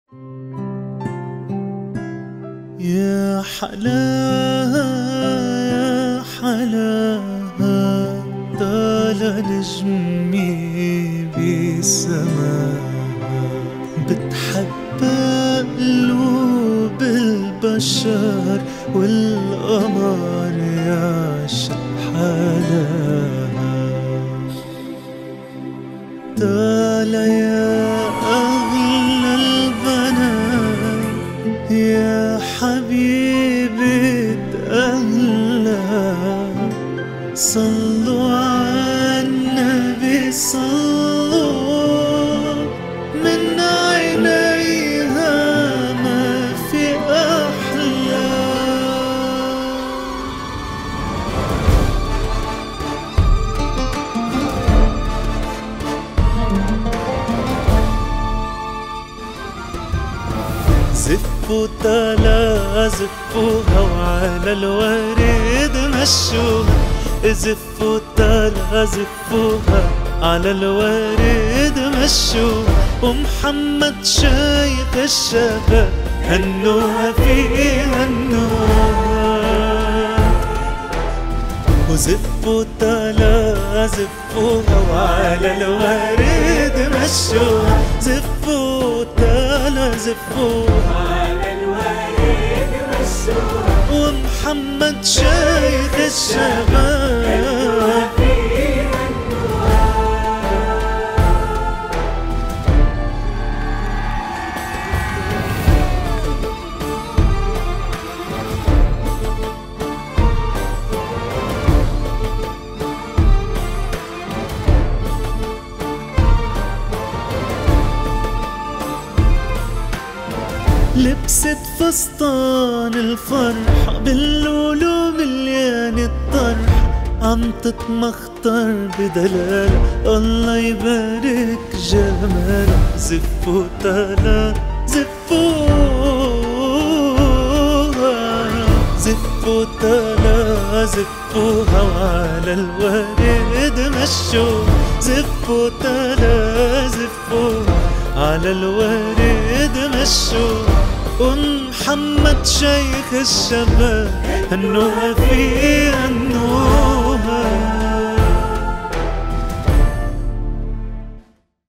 SOUND ON ! Beautiful Customized Wedding Song for This Gorgeous Bride !
Nothing is more special than walking into your wedding on a customized touching song !
Watch this bride making an entry wearing a Show-stopping fully embroidered wedding gown.